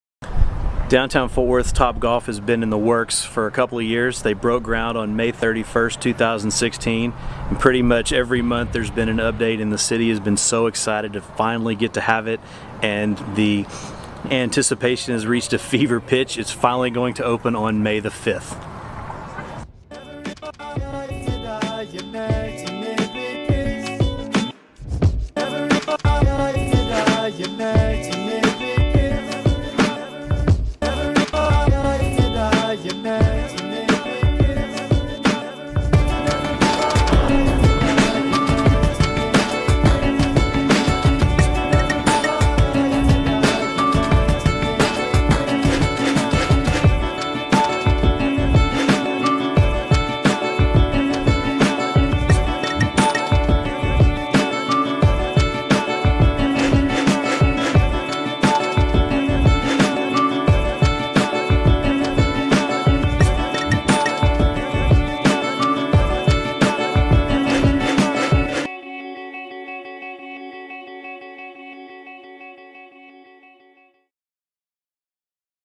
Check out the inside of a Topgolf
Here's a sneak peek at the driving range and lounge of a new Topgolf in Fort Worth, Texas.